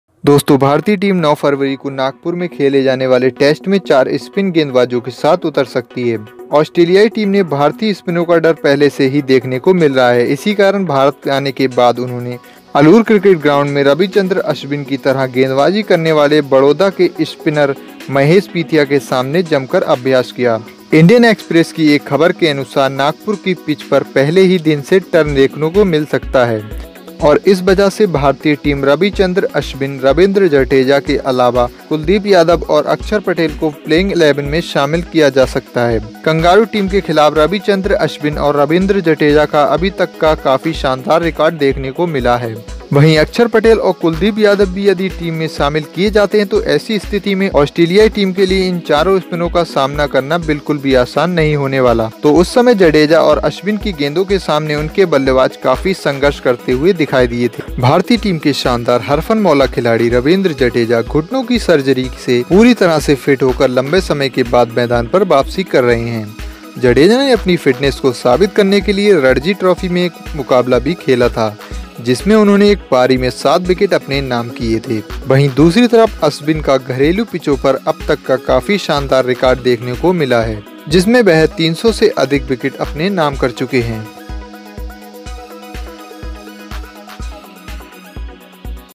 What is India's strategy regarding the Nagpur Test?
In such a situation, now a shocking news is coming out. In fact, according to the report, the Indian team can field 4 spin bowlers in the Test match to be played in Nagpur on February 9. The fear of Indian spinners is already being seen in the Australian team. For this reason, after coming to India, he practiced fiercely in front of Baroda spinner Mahesh Pithia, who bowled like Ravichandran Ashwin Alur Cricket Ground. Apart from Ravichandran Ashwin, Ravindra Jadeja, Kuldeep Yadav and Akshar Patel can be included in the playing eleven of the Indian team.